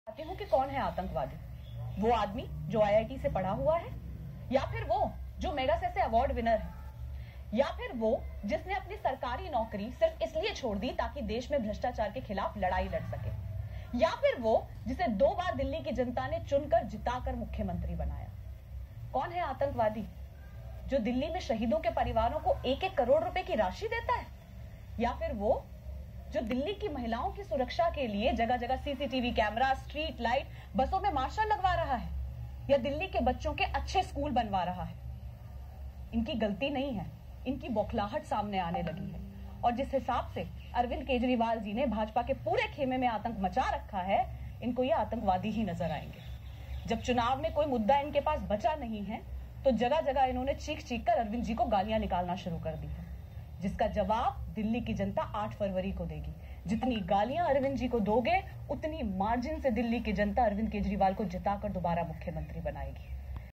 BJP calls Arvind Kejriwal Delhi CM a Terrorist
he says he’s Delhi’s bhai n beta
who’s more convincing
Delhi CM Arvind Kejriwal hit back at BJP leader Parvesh Verma for calling him a "terrorist", saying he had worked like the son and brother of Delhi
residents in the past five years.